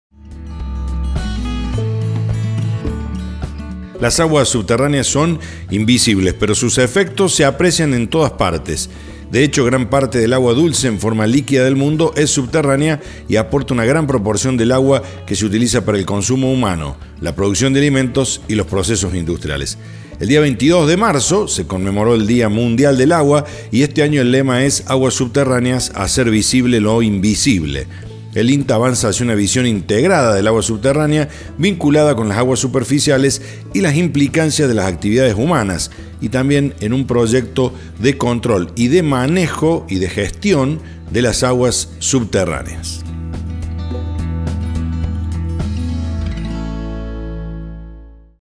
EL AGUA MAS VALIOSA
Las aguas subterráneas son invisibles, pero sus efectos se aprecian en todas partes. De hecho, gran parte del agua dulce en forma líquida del mundo es subterránea y aporta una gran proporción del agua dulce que se utiliza para el consumo humano, la producción de alimentos y los procesos industriales. El 22 de marzo se conmemora el Día Mundial del Agua y este año el lema es "Aguas subterráneas: hacer visible lo invisible". El INTA avanza hacia una visión integral del agua subterránea, vinculada con las aguas superficiales y las implicancias de las actividades humanas.